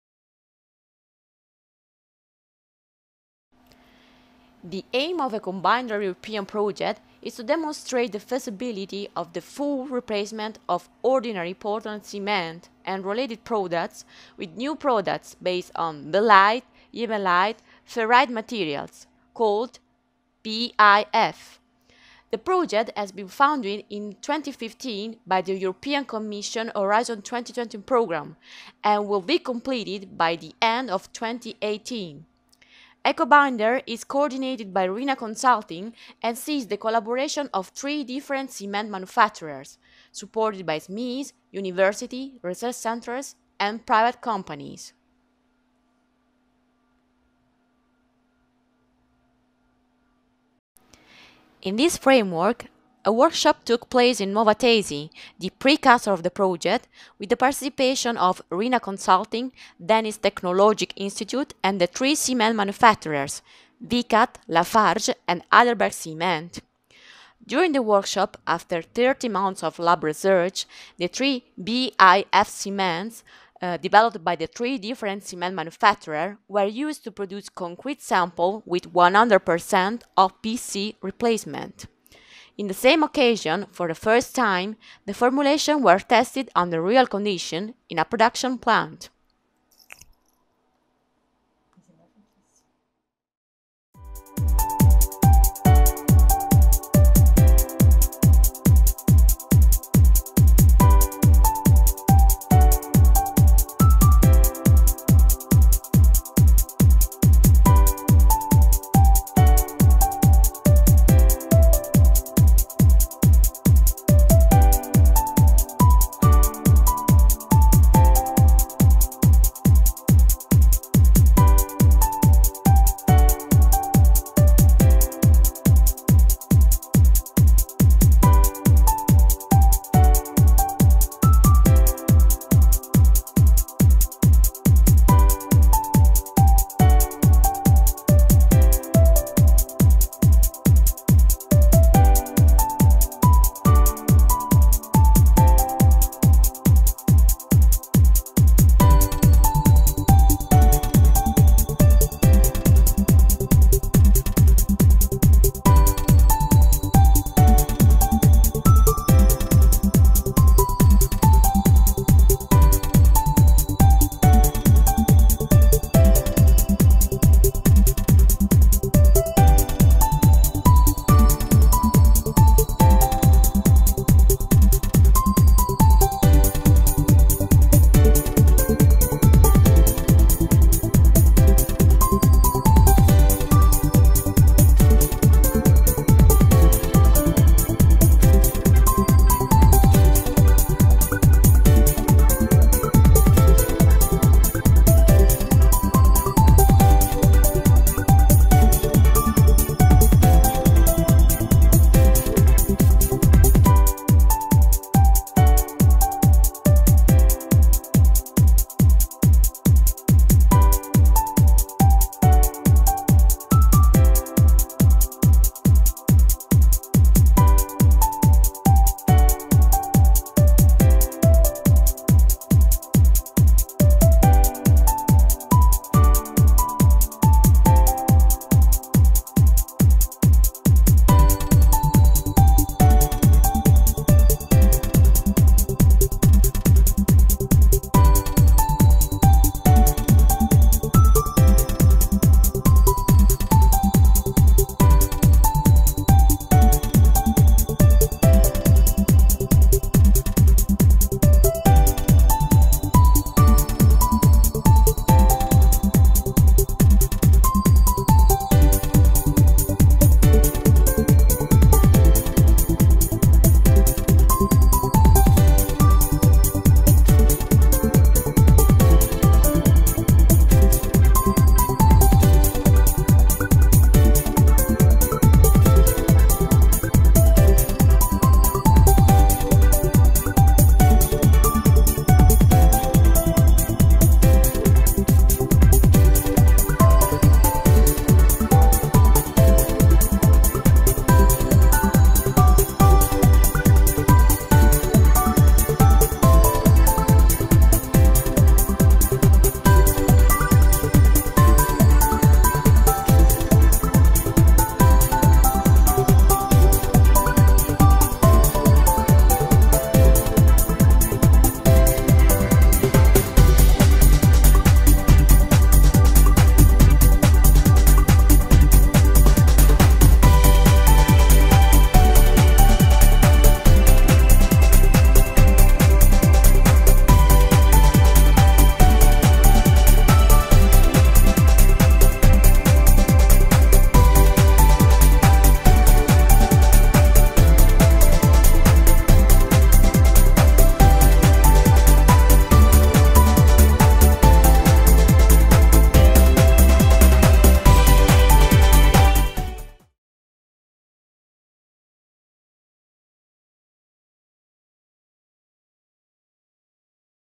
Development of panels at Nuova Tesi premises I ECO-Binder Project
ECO-BINDER is a European research project under H2020 programme. The main objective of the ECO-Binder project is to demonstrate the feasibility of replacing Ordinary Portland Cement (OPC) and OPC based concrete (products) with new products based on the innovative Belite-Ye’elimite-Ferrite (BYF) class of low-CO2 binders.
The video was created by RINA.